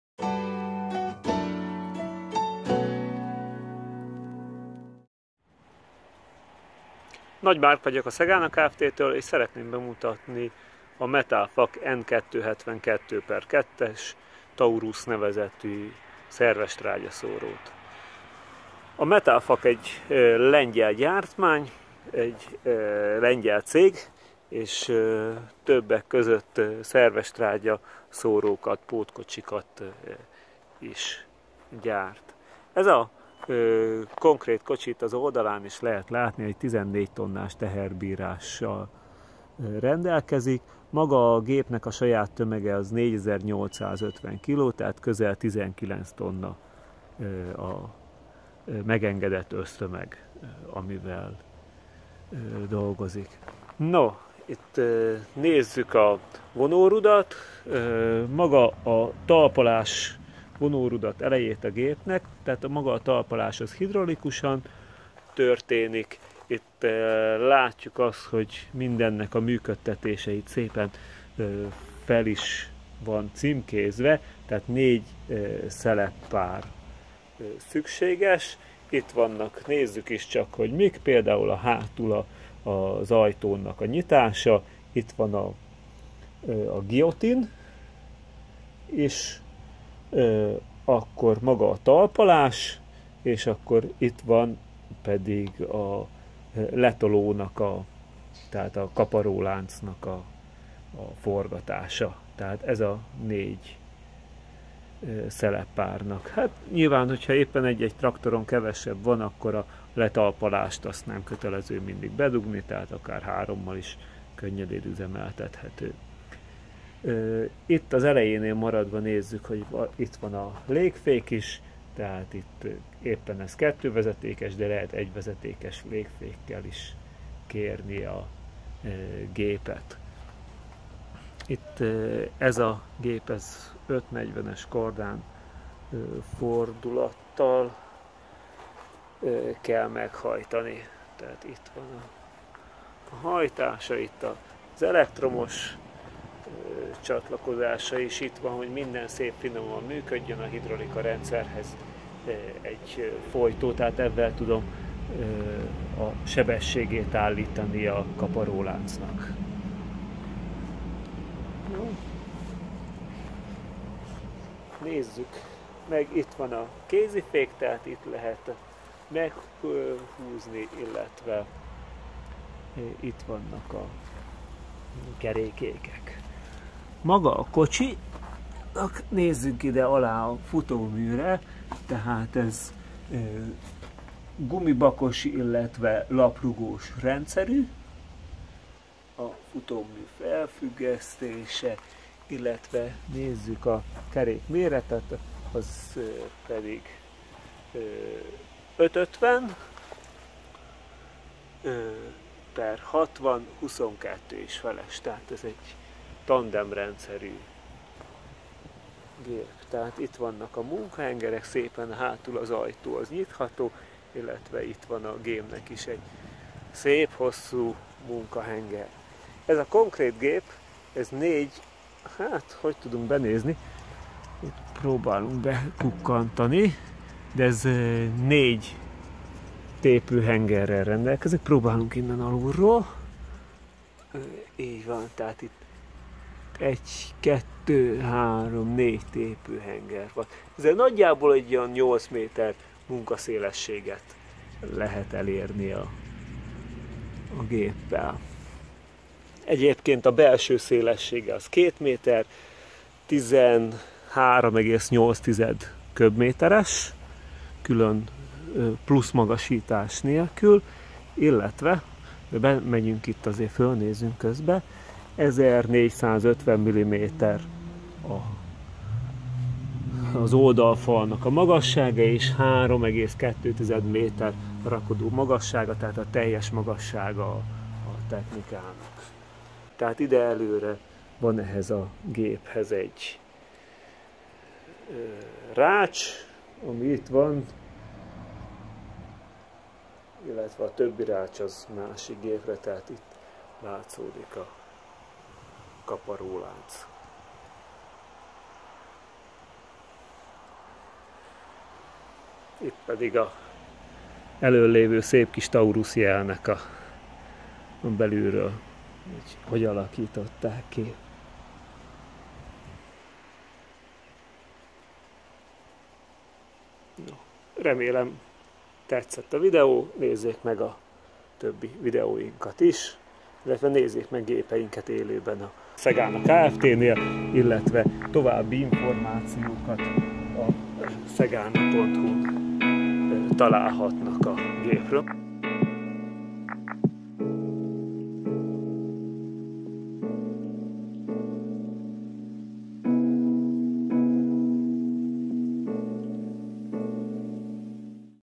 Szegána - Metal-Fach N272/2 szervestrágya szórókocsi bemutató
Metal-Fach N272-2 szervestrágya szórókocsi magyar nyelvű bemutató.
A szervestrágya szórókocsi megtalálható a 6791 Szeged, Dorozsmai út 143. szám alatt található szegána Kft. telephelyén.